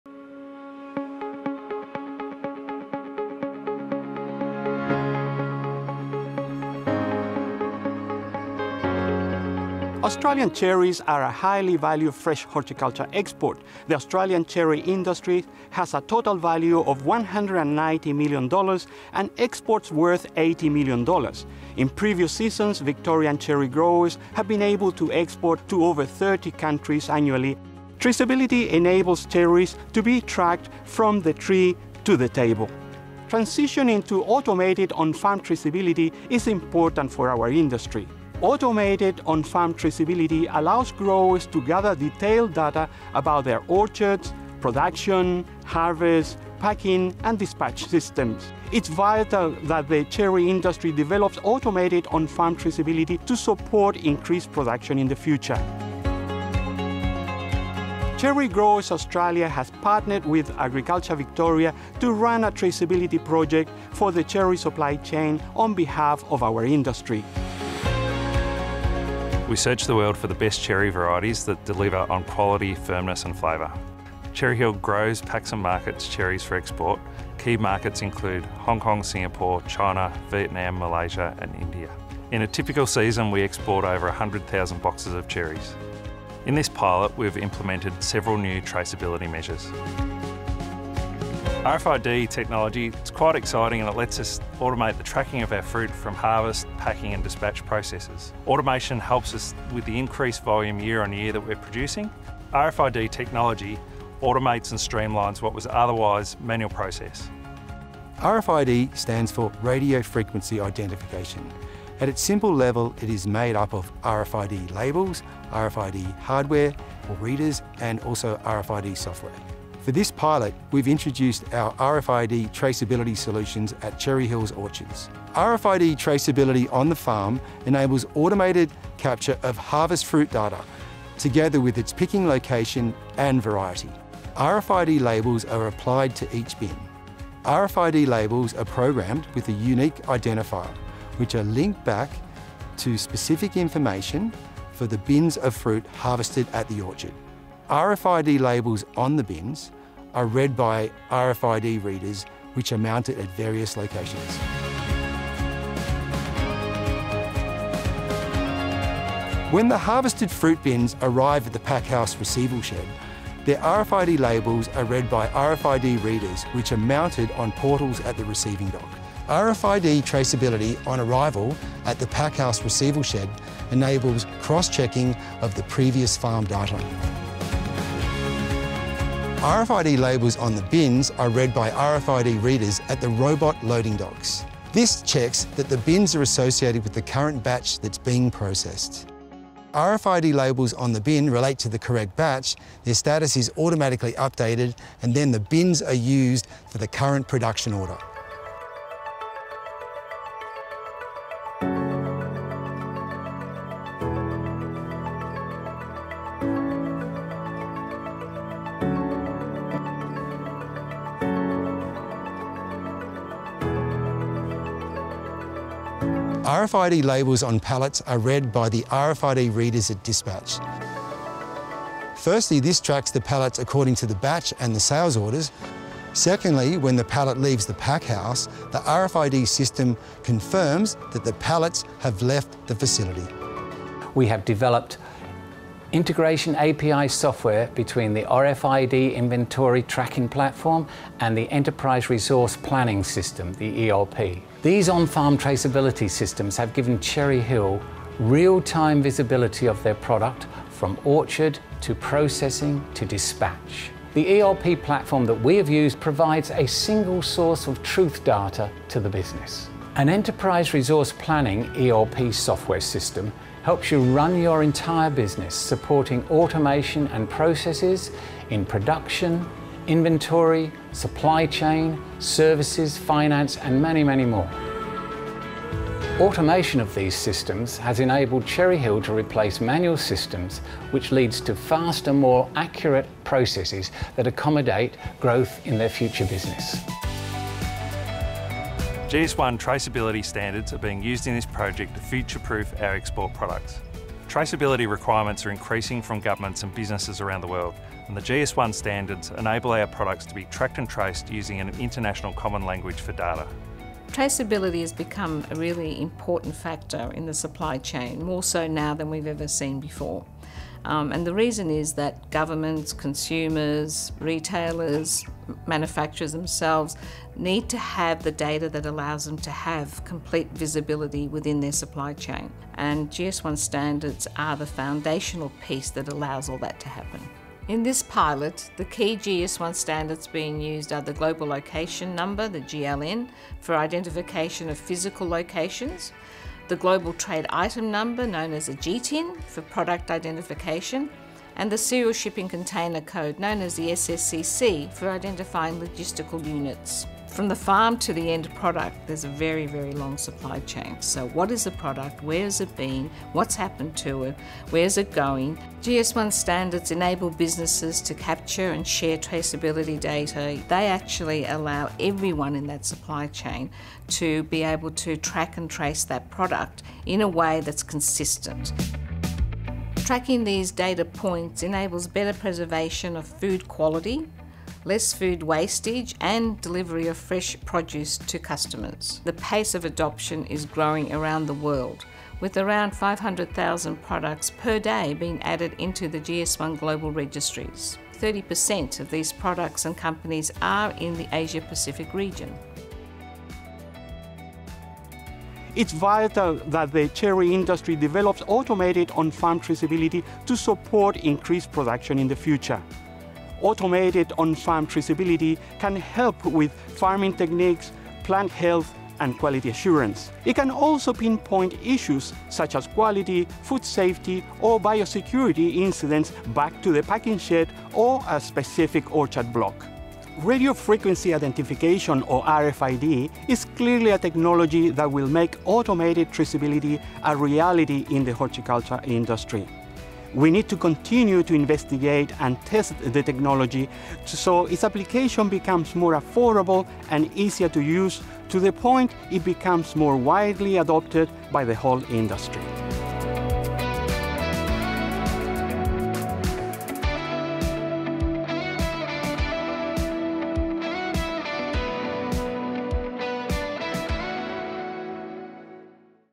Explore RFID Technology in Cherry Production - Australian Cherries Traceability Pilot Project
Join us in uncovering the future of cherry production with the RFID technology that was used in the Cherries Traceability Pilot.

Our latest video dives deep into what RFID technology is, and the implementation at CherryHill Orchards revolutionising how you track and manage your cherries from orchard to table.

Discover the importance of traceability in the cherry industry, the transition to automated on-farm systems, and the role of RFID technology in streamlining processes and ensuring premium quality is monitored efficiently.

Watch now to learn from industry experts about, the integration of RFID inventory tracking with the enterprise resource planning (ERP) system and the adoption of GS1 traceability standards.

Embrace innovation and sustainability as we pave the way for a more transparent and efficient future. 🍒🌱